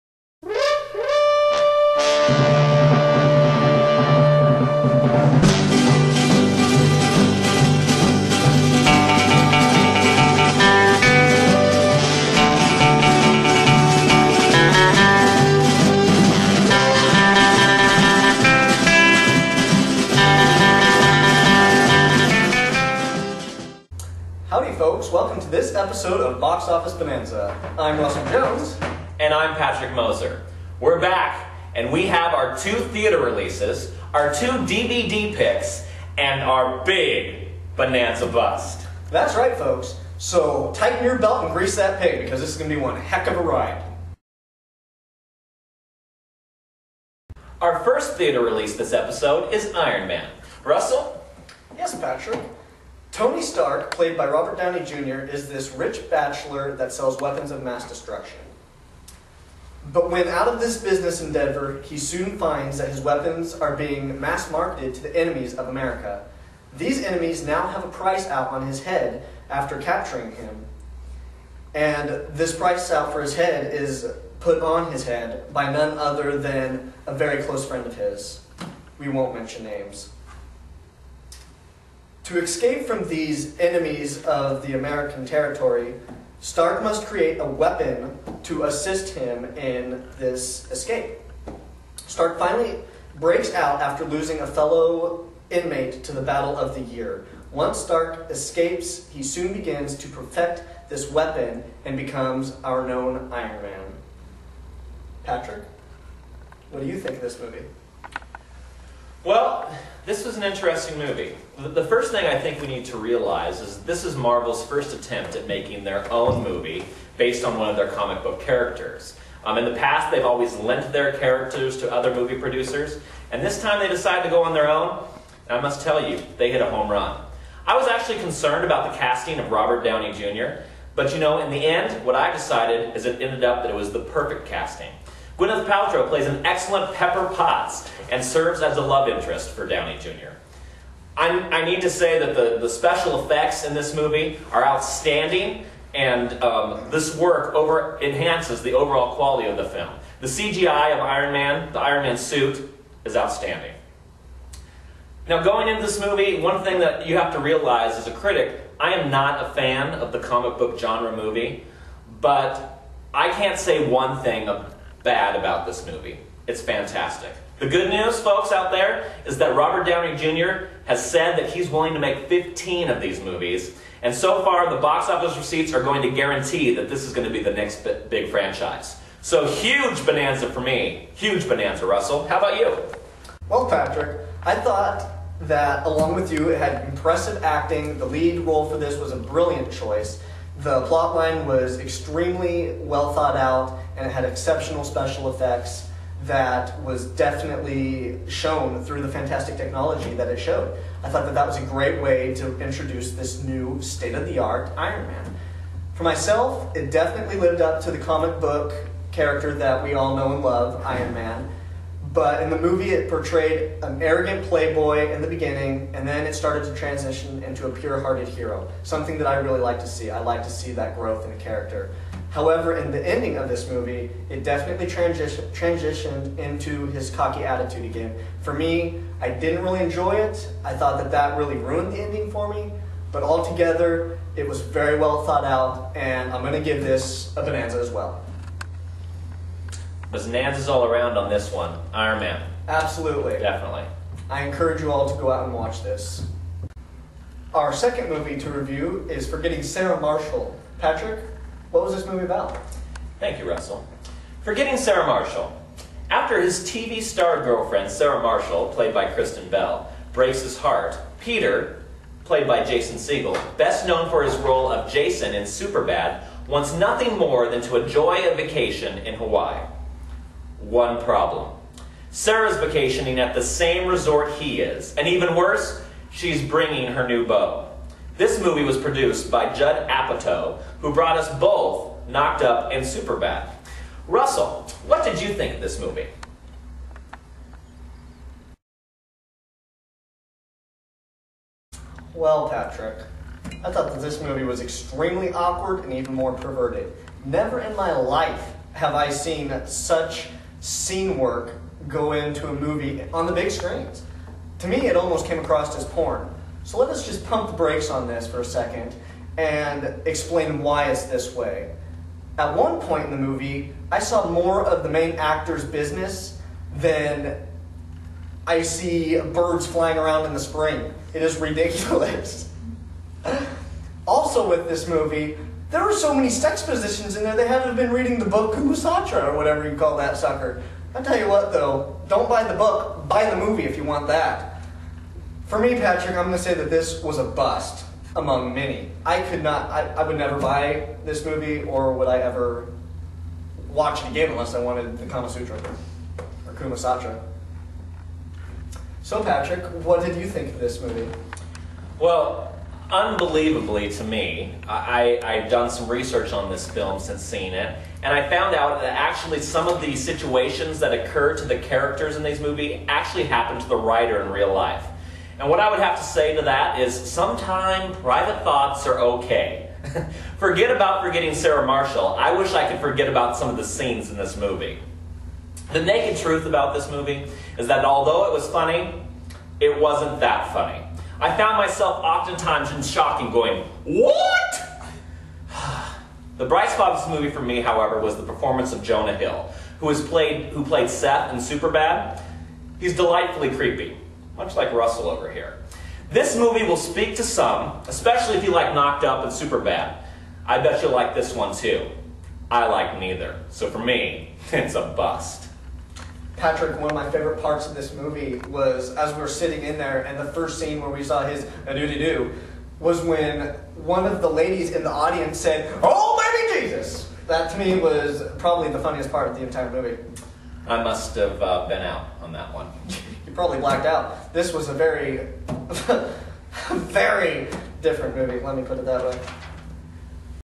Box Office Bonanza Episode 3 Part 1
In this episode we review Iron Man, Forgetting Sarah Marshall, August Rush, The Great Debaters, and we end it all up with Cloverfield.

We hope your next trip to the box office is a bonanza!